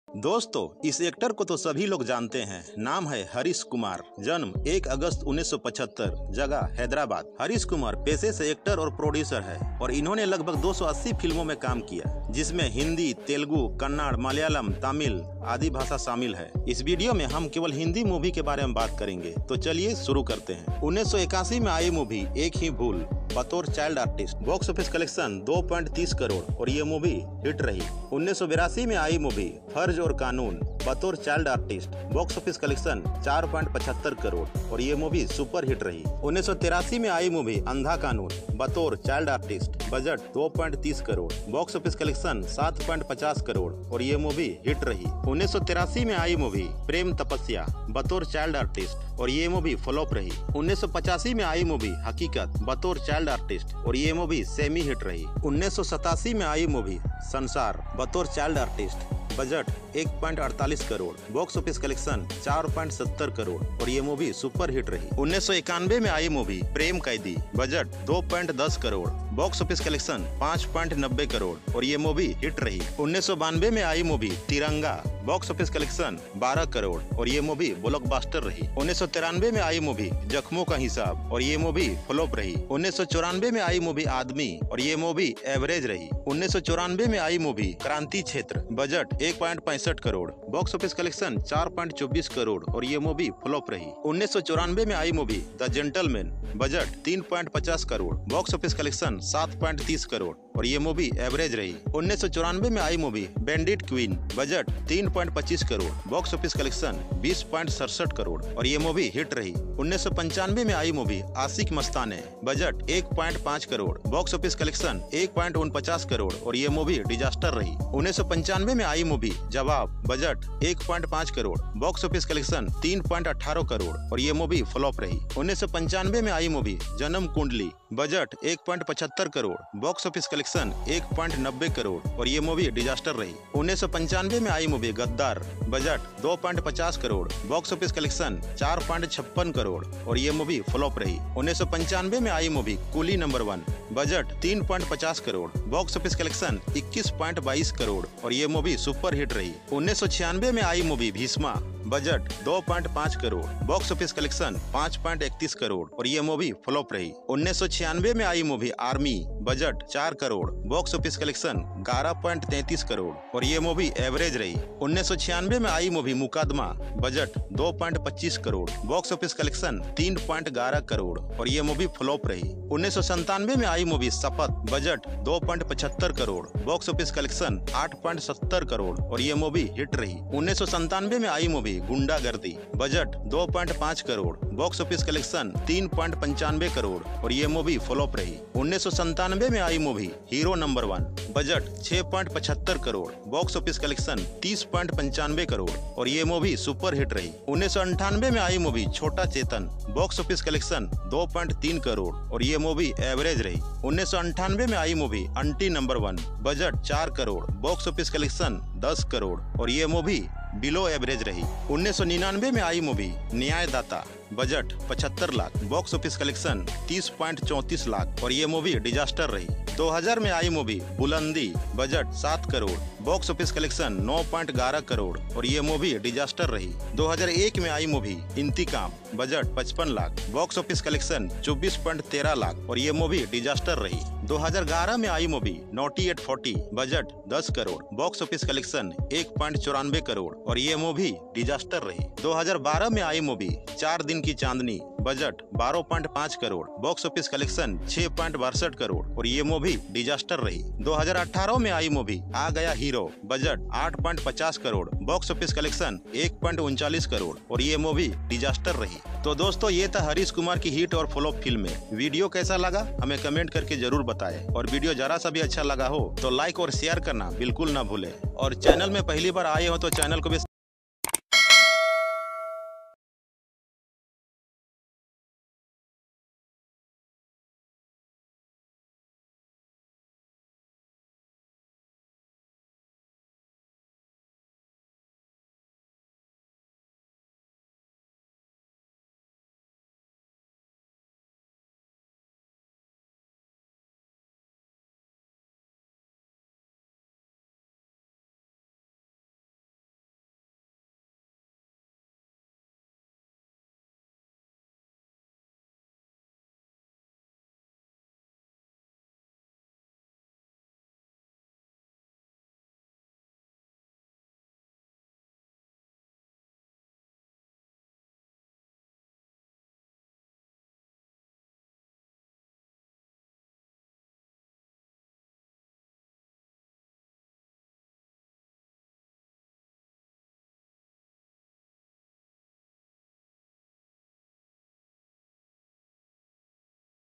Harish Kumar (1981-2022) All Movies List | हरीश कुमार के सभी हिट/फ्लॉप फ़िल्में | Harish Kumar Movie
Dosto is video me maine harish kumar ke sabhi hindi filmo ke bare me bataya hai. asha krta hun, aplogo ko ye video jarur pasand aya hoga. video ko like, share bilkul na bhule.

Copyright